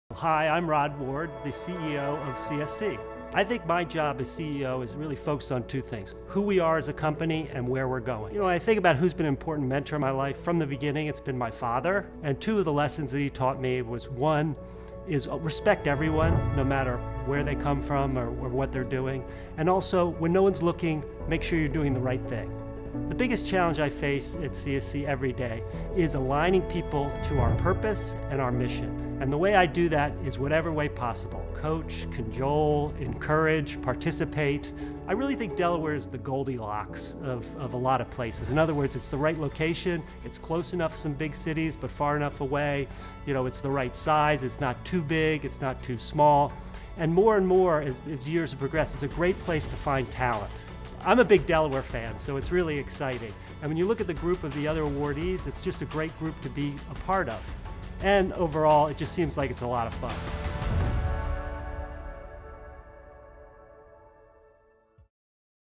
Rod Ward III - CEO of the Year 2021
Rod Ward III is the President and CEO of CSC and winner of CEO of the Year in the Large For-Profit category.